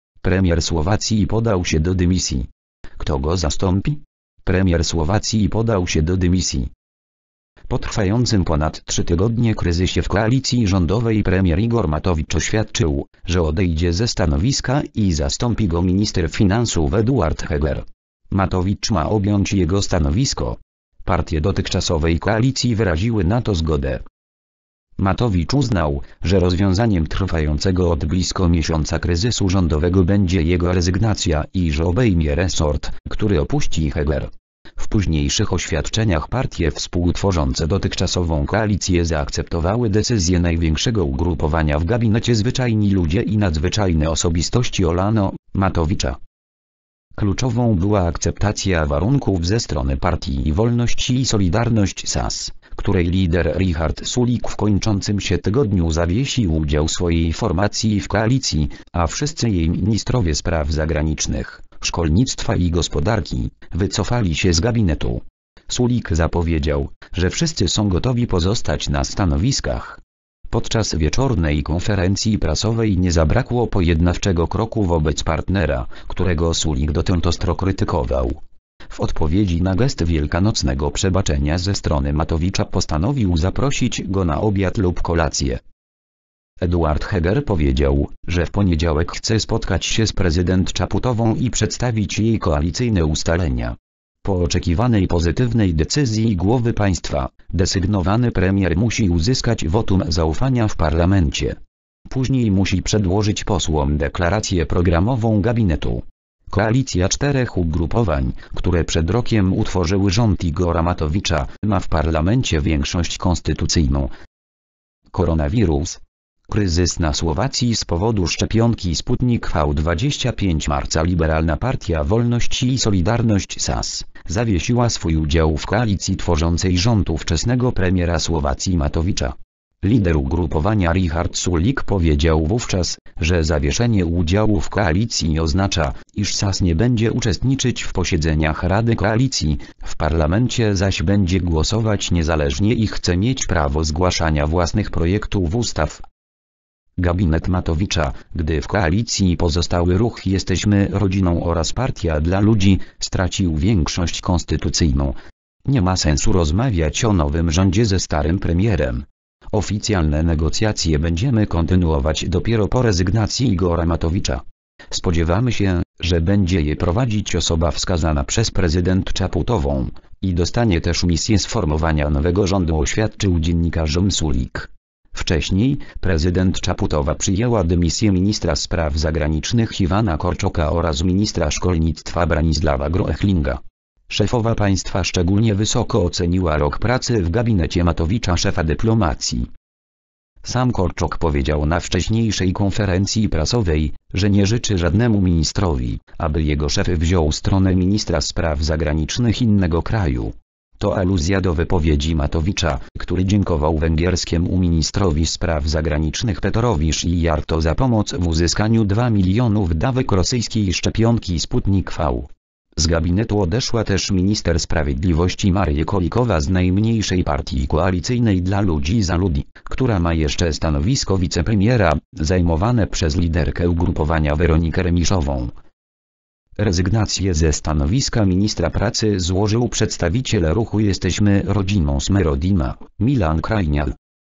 Premier Słowacji podał się do dymisji. Kto go zastąpi?
Premier Słowacji podał się do dymisji.

   (fot. PAP/EPA/CHRISTIAN BRUNA)